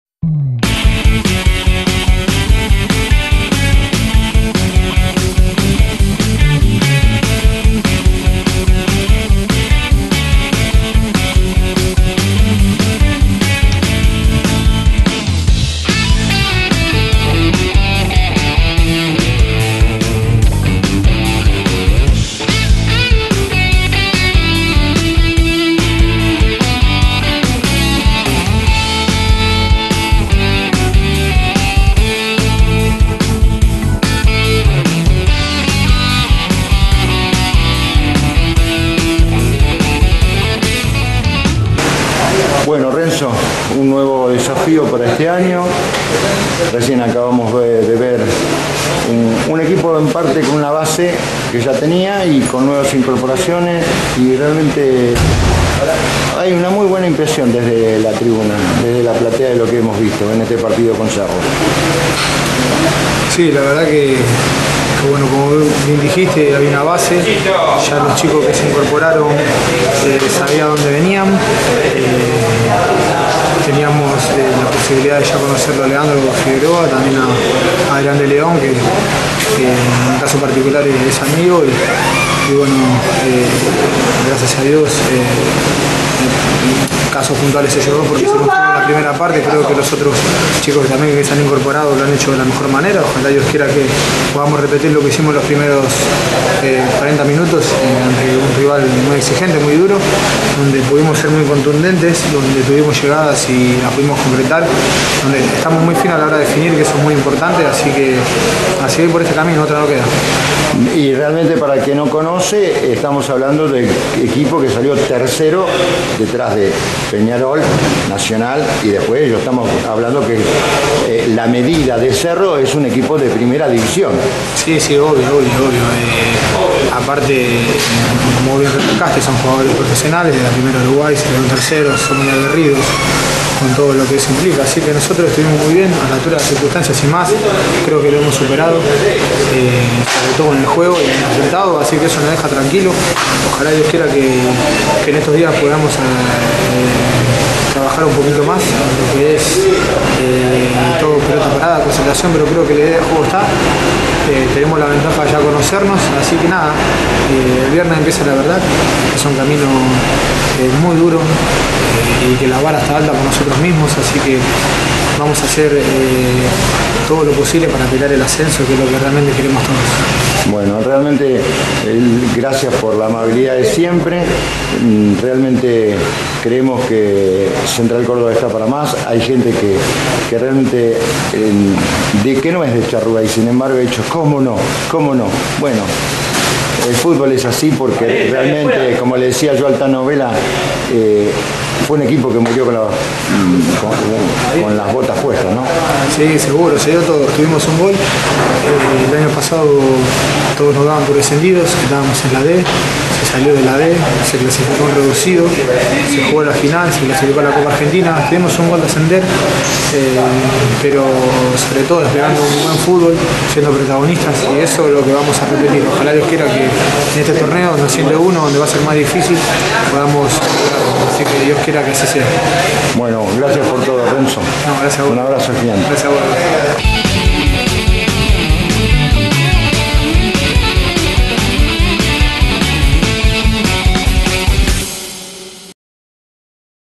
Renzo Funes- Luego del triunfo a Cerro
Nota a Renzo Funes luego del triunfo a Cerro de Uruguay por 3 a 2